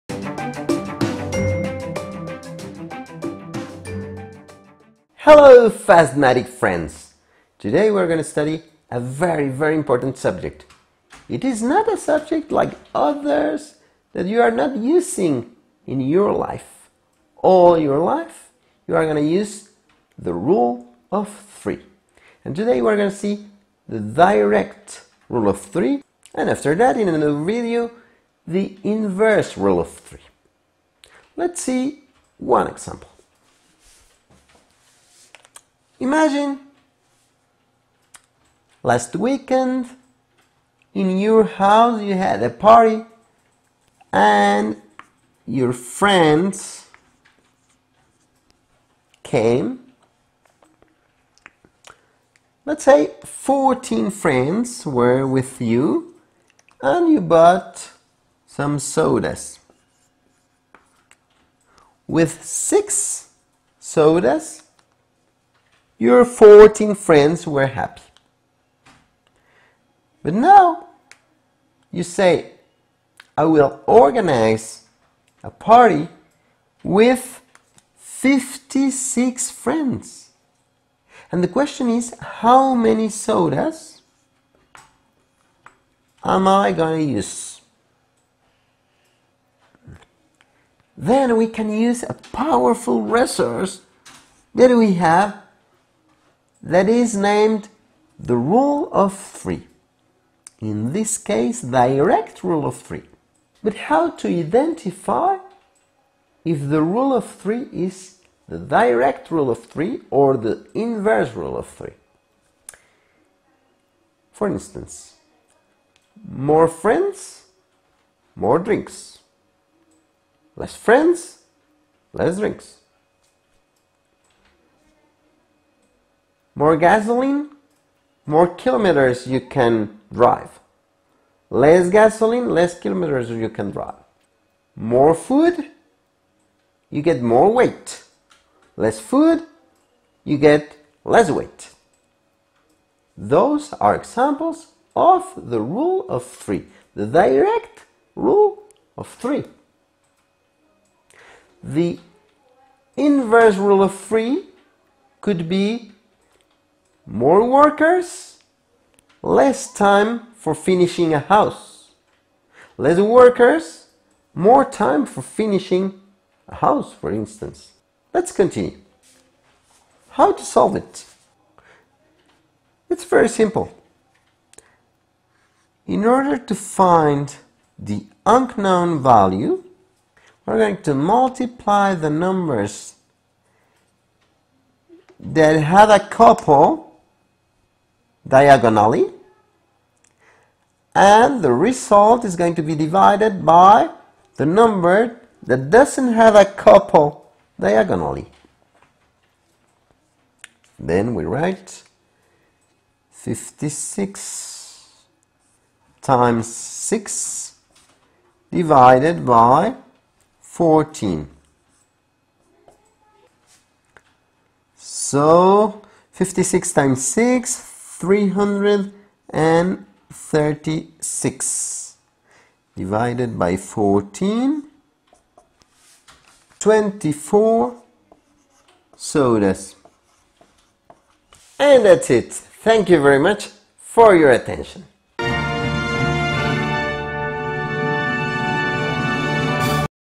32. ARITHMETIC: Ratios and proportions (direct rule of three)
Learn hoe to solve problems with direct rule of three.